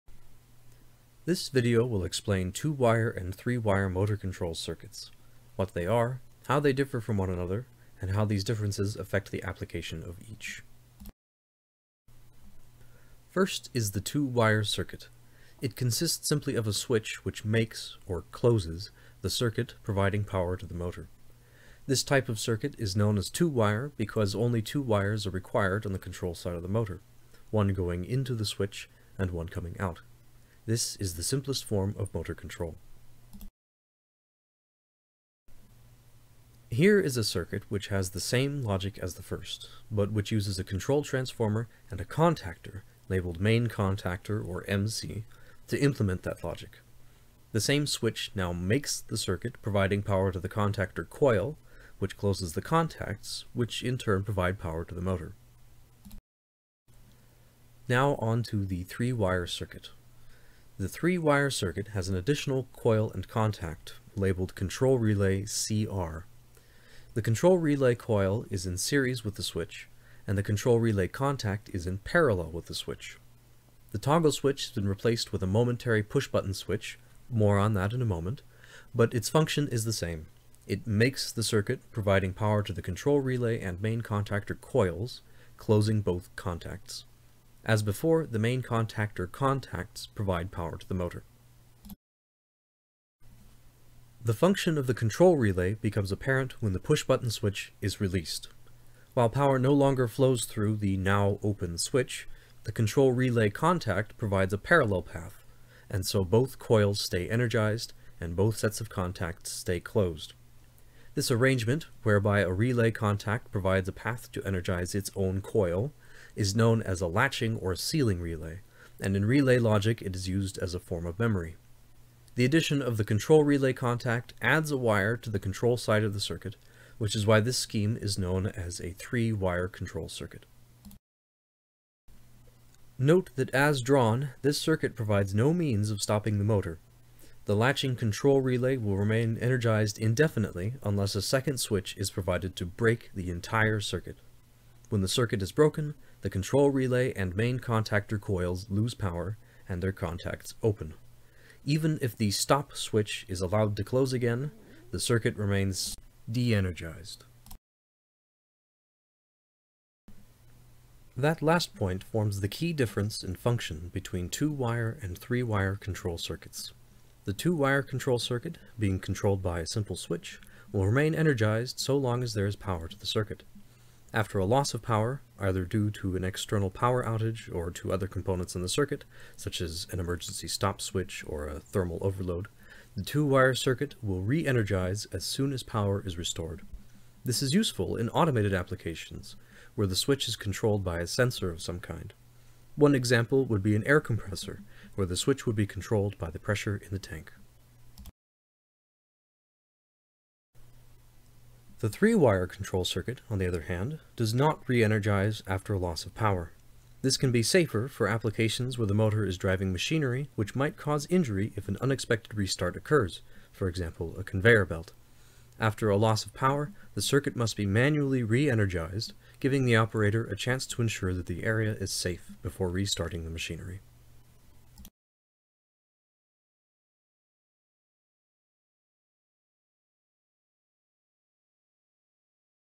Two Wire and Three Wire Motor Control Circuits
- What they are
- The differences between them
- How these differences affect the applications of each